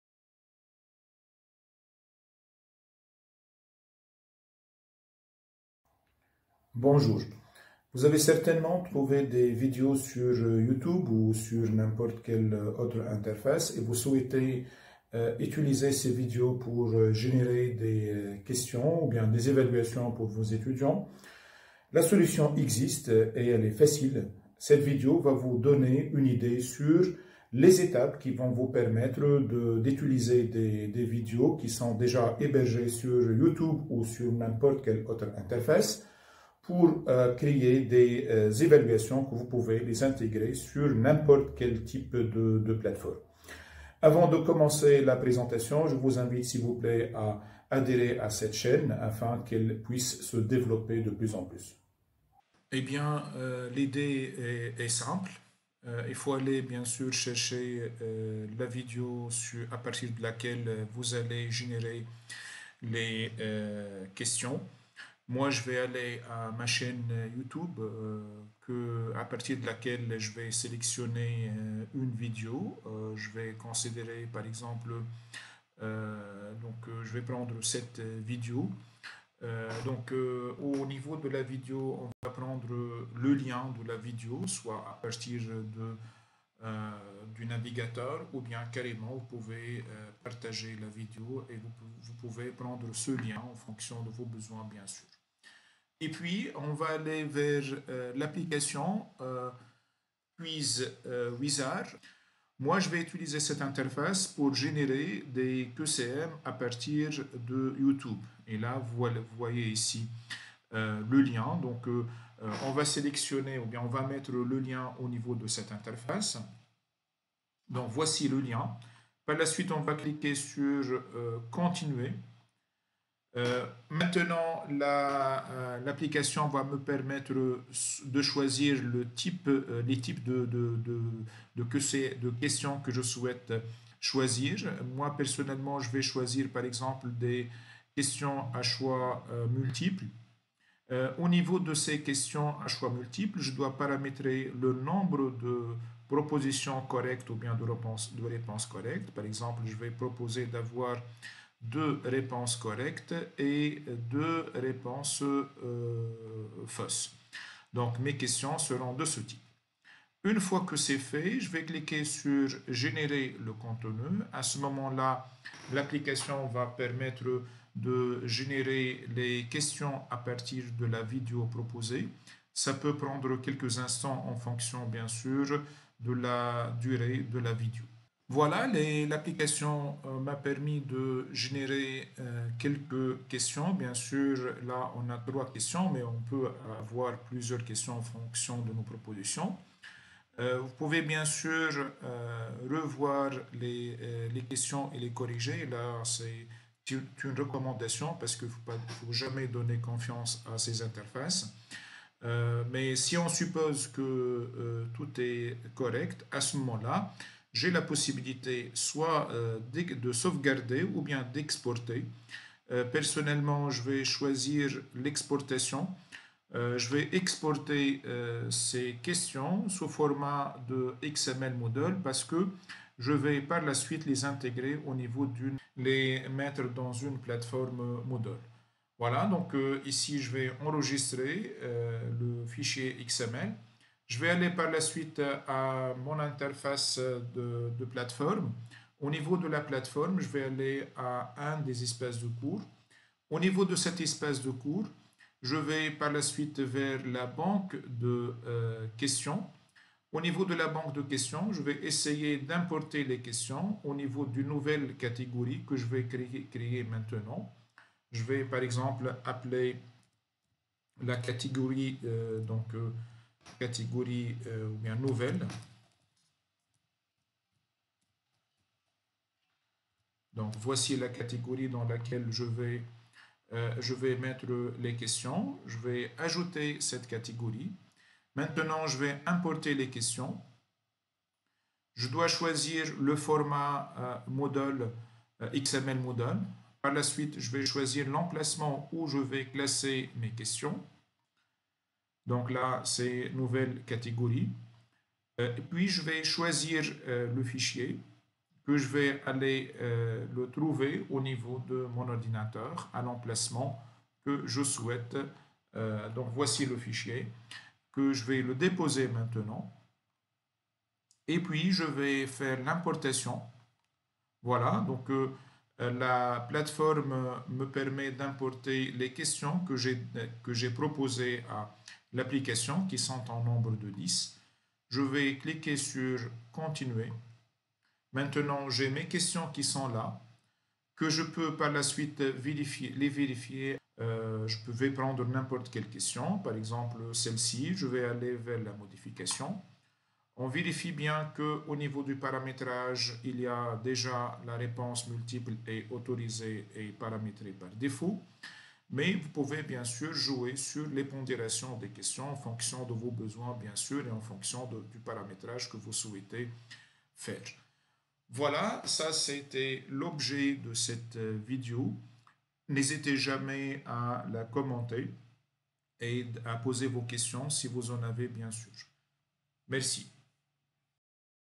Comment générer des QCM depuis une vidéo sur
Cette vidéo explique les étapes à suivre afin de générer des QCM depuis une vidéo hébergée sur YouTube